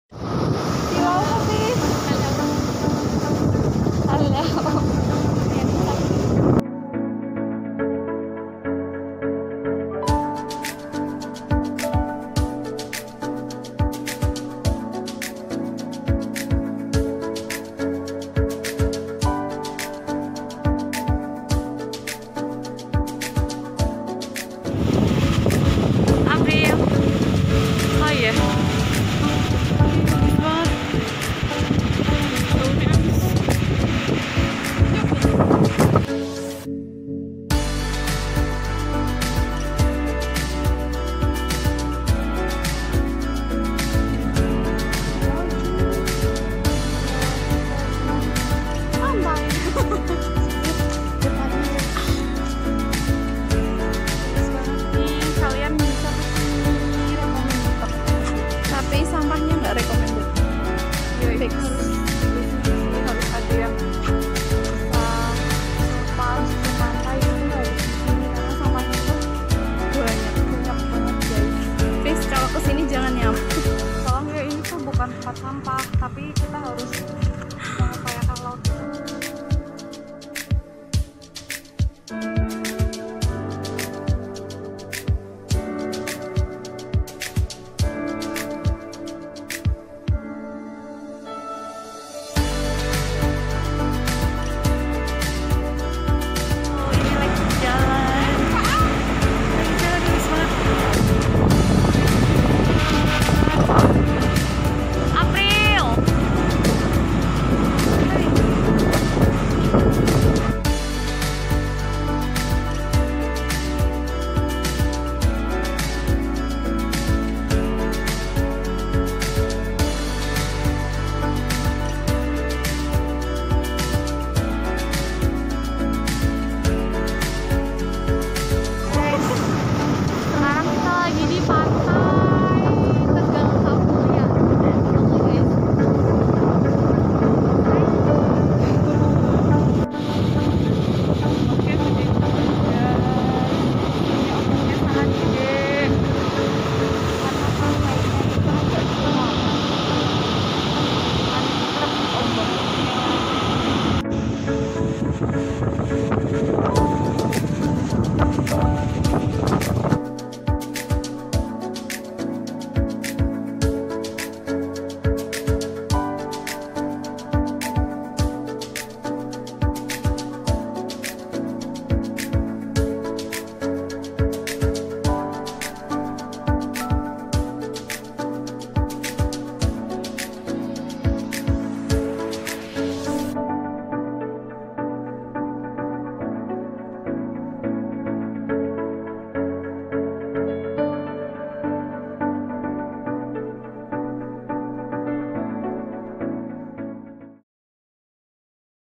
Pantai Ketapang Dengklok, Tegalkamulyan, Cilacap [Vlog by Nilna & April]
Video vloging dari teman kita Nilna & April mengenai kunjungannya ke Pantai Ketapang Dengklok, Tegalkamulyan, Cilacap.